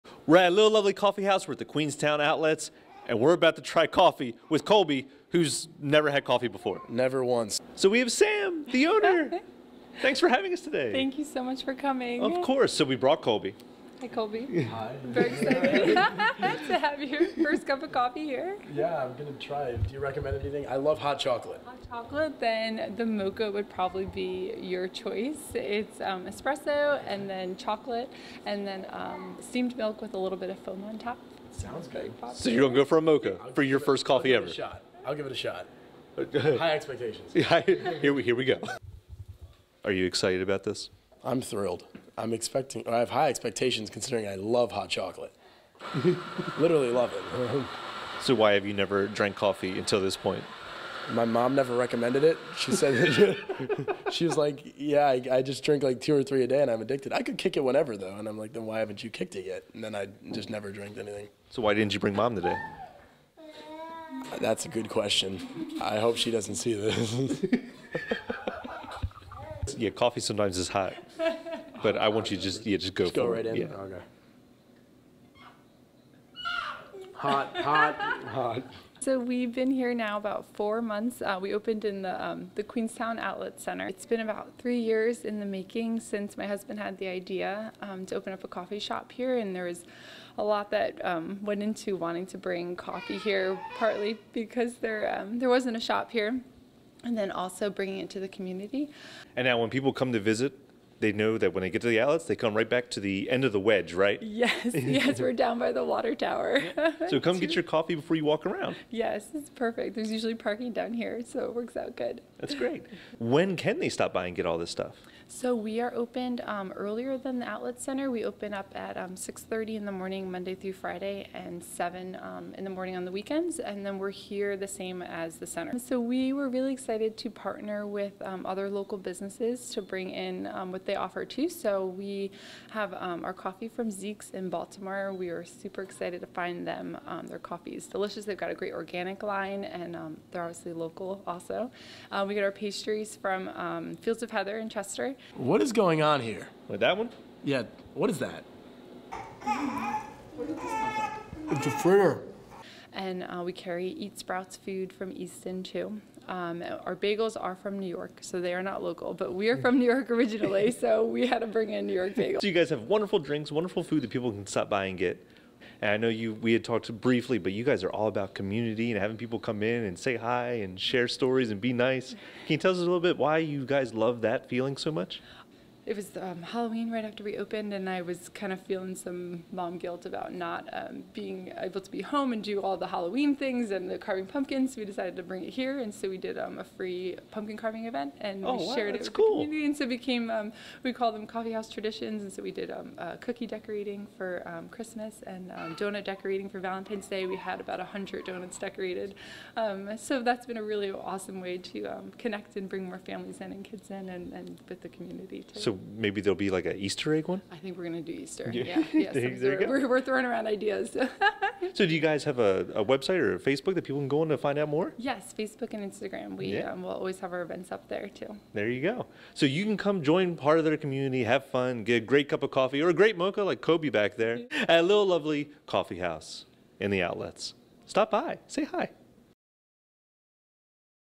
A Little Lovely Coffee House
A Little Lovely Coffee House is serving up delicious drinks and kindness at the Queenstown Outlets in Queen Anne's County, Maryland. Bruce Grove stops by with our intern Colby to get him his first coffee drink ever and meet the owner Samantha.